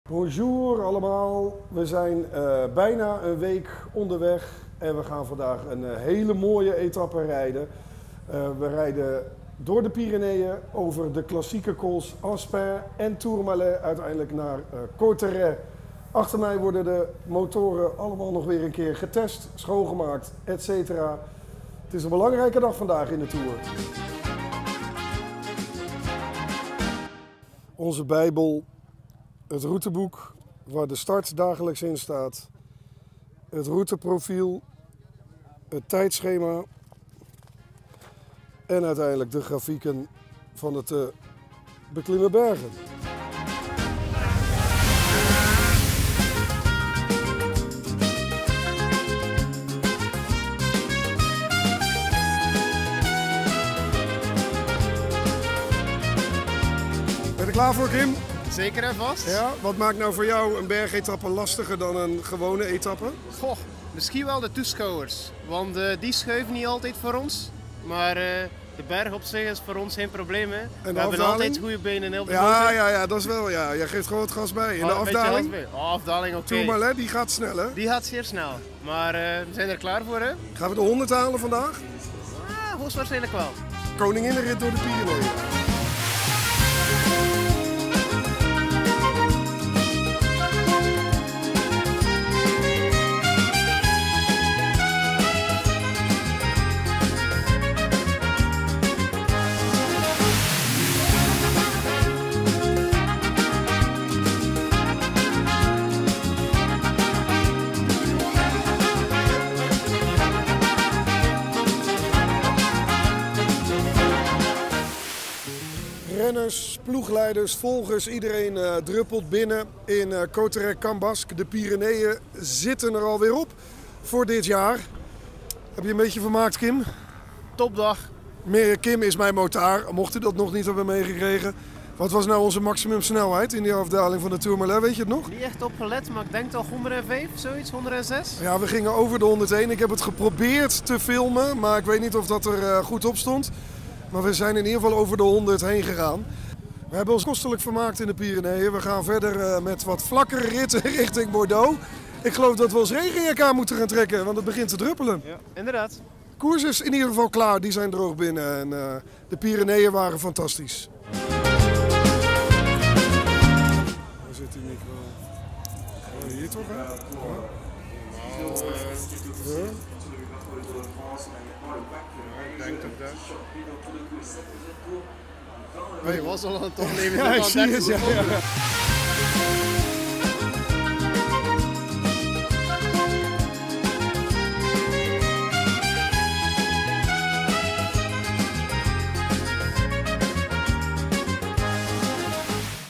De Pyreneeën door met Sebastiaan Timmerman | Radio Tour de France
NOS-commentator en Onze Man op de Motor neemt je deze keer mee door de Pyreneeën van de Tour de France.

De bergetappes gaan door de prachtige Pyreneeën, die ook voor motard Kim altijd een extra uitdaging vormen. Met meer dan 100 km/h doet Sebastiaan verslag van de etappes voor Radio Tour de France.